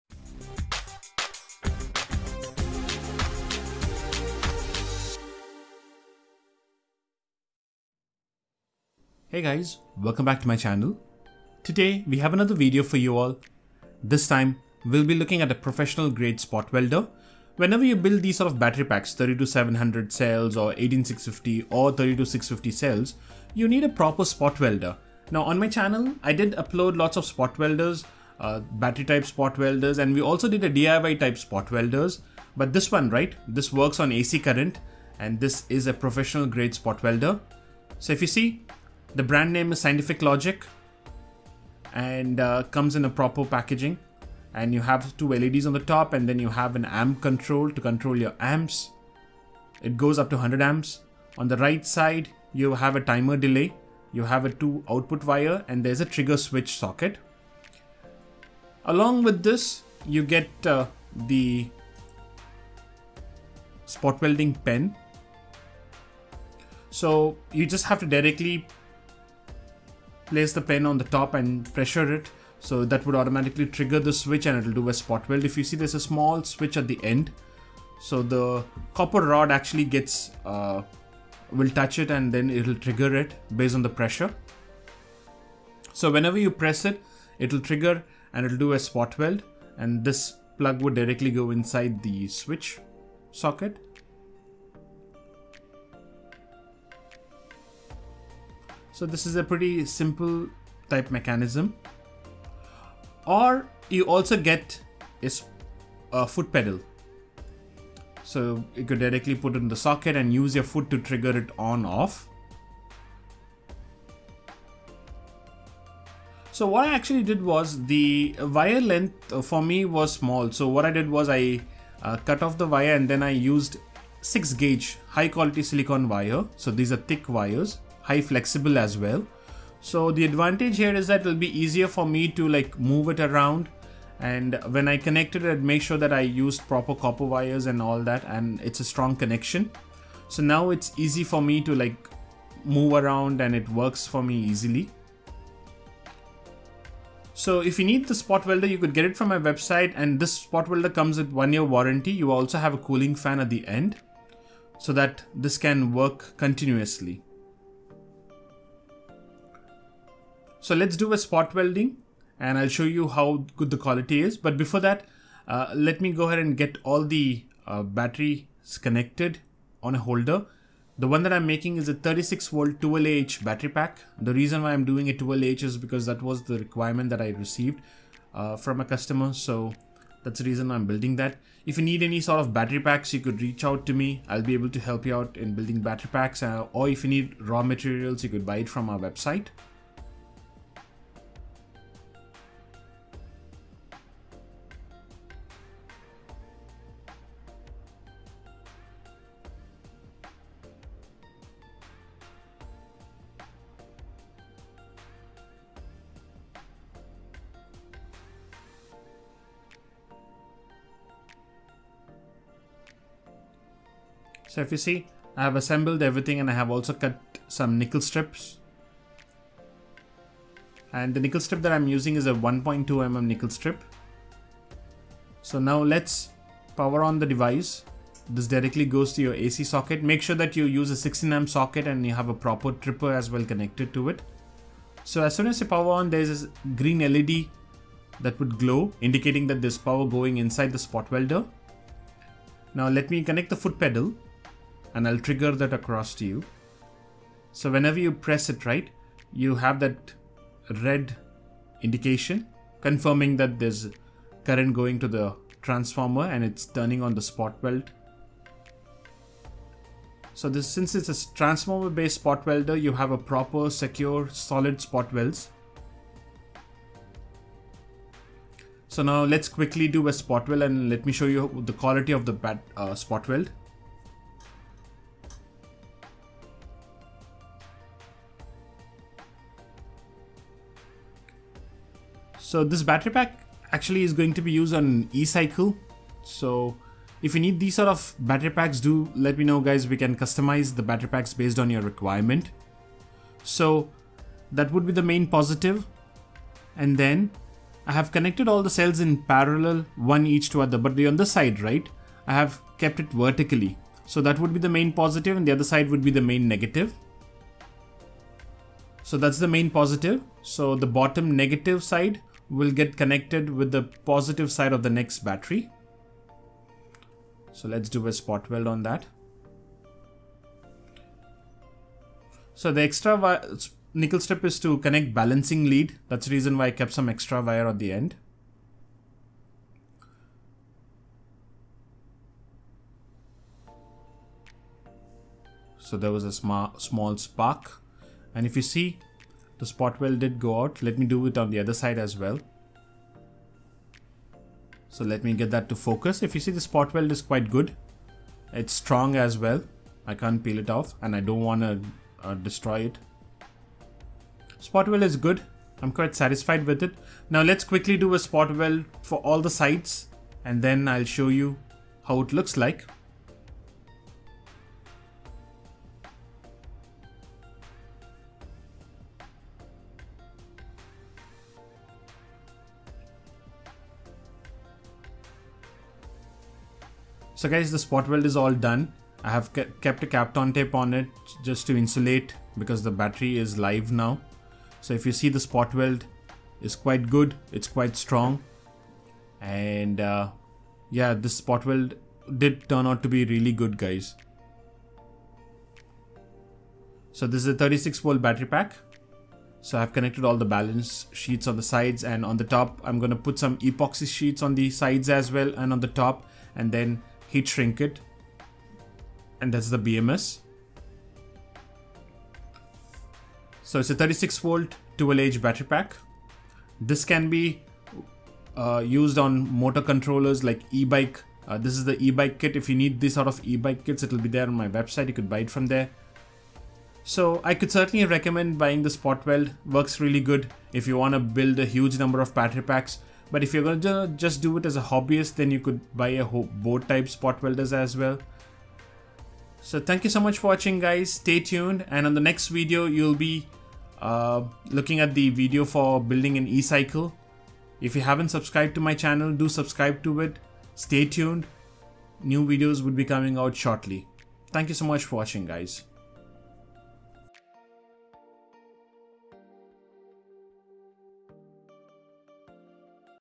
230V AC Lithium Battery Spot Welder- Transformer Based Large battery packs
in this video, we are taking a look at the 230v AC based lithium battery spot welder. This can be used to build large number of battery packs and will support continuous usage. Comes with 1 year warranty from the manufacturer. Please make sure that you connect it in a 16A socket and the fuse is also proper sized.

230v 10A AC input
Output 40A to 100A
Spot weld 1mm to 1.5mm nickel coated strips
do not use in thick nickel strips
brand Scientific Logic